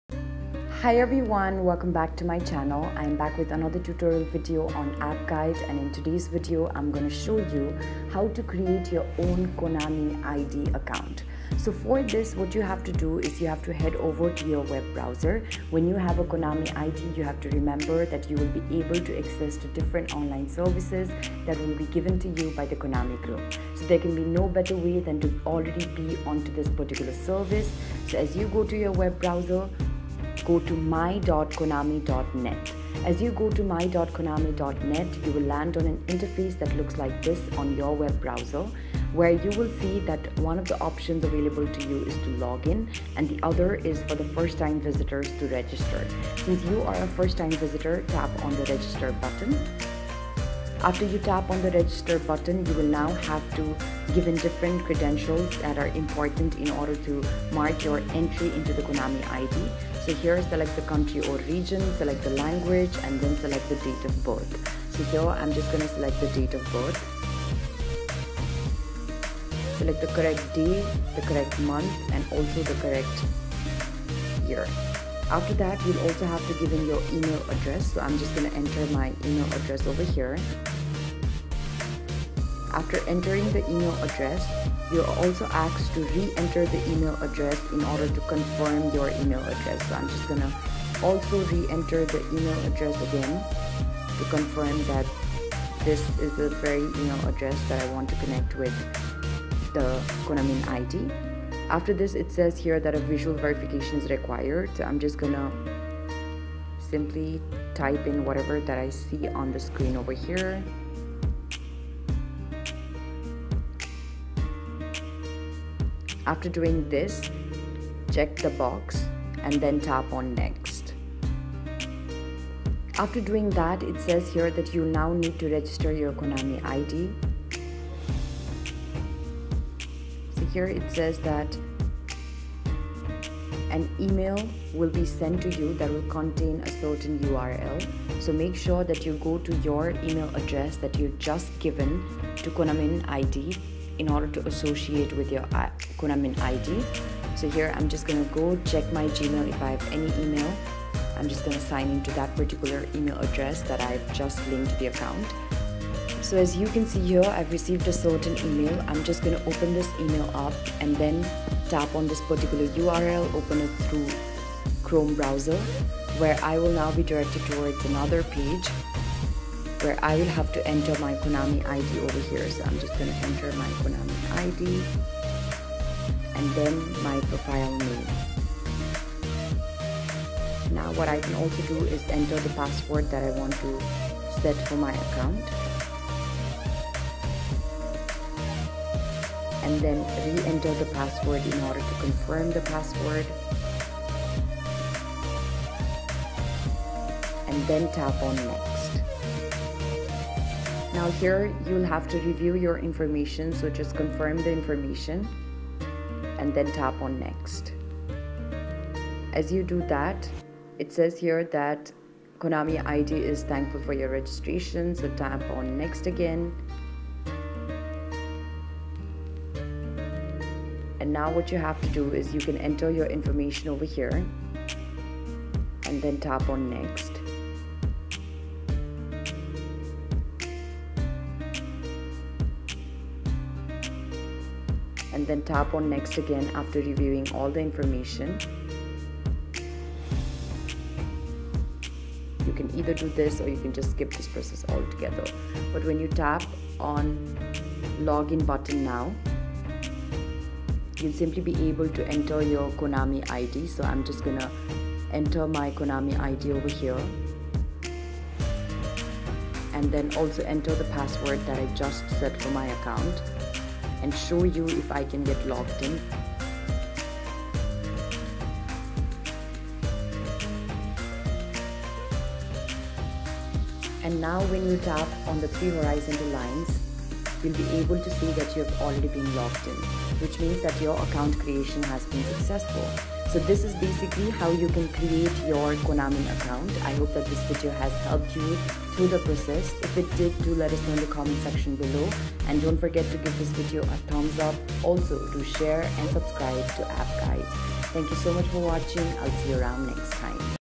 How to Create KONAMI ID | Register your KONAMI Account - My KONAMI 2021
➜ Have you been wondering on how you can create Konomi id account? This video guides you in easy step by step process on how you can create Konomi id account.

Follow these simple steps:

1.Open a browser and go to "my.konami.net"
2.Tap on register.
3.Fill out all the information.
4.Tap on the link sent to your email by konami.
5.Register your account.
6.And you're all done.